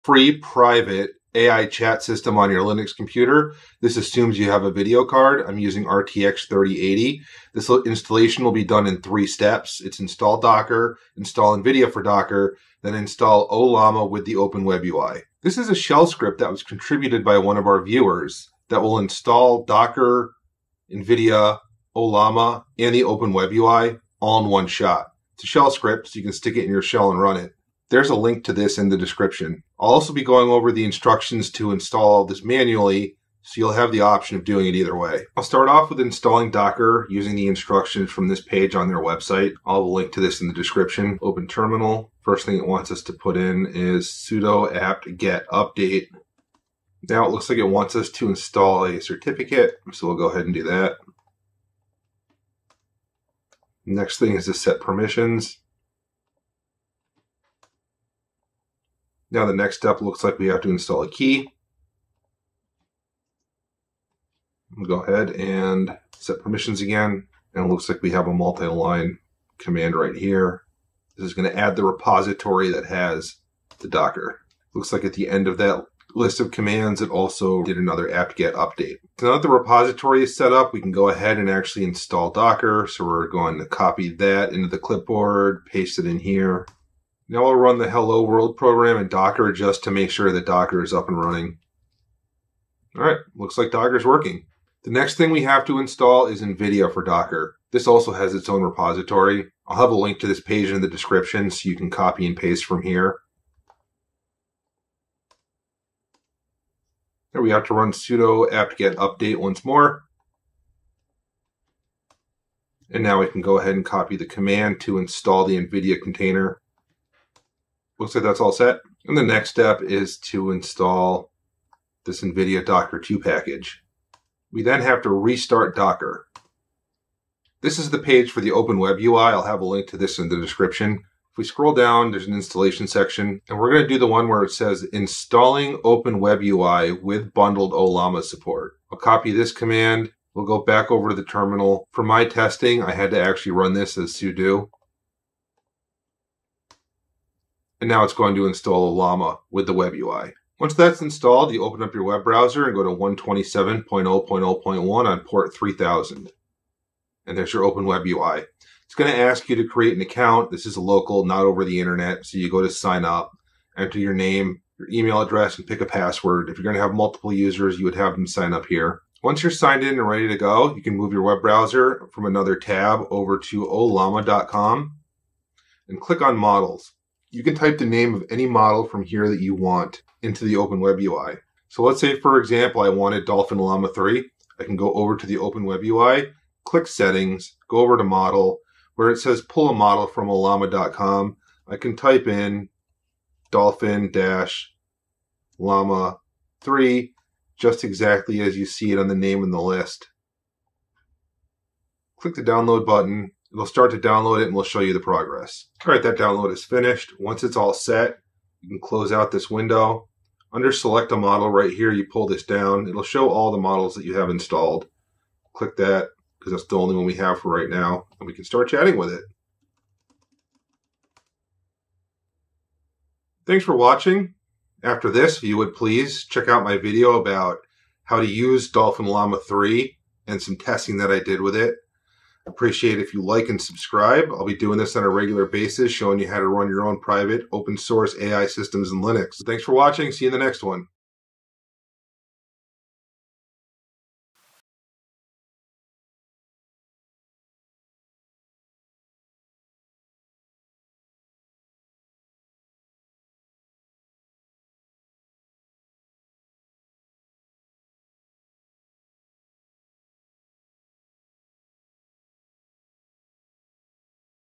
How to Run Local AI Chat On Linux Using Ollama, Quick and Easy
Get up and chatting with your own local AI. Download a variety of large language models. Chat offline and privately using a clean interface.

Step 3, Install nvidia-docker2:
sudo apt install nvidia-docker2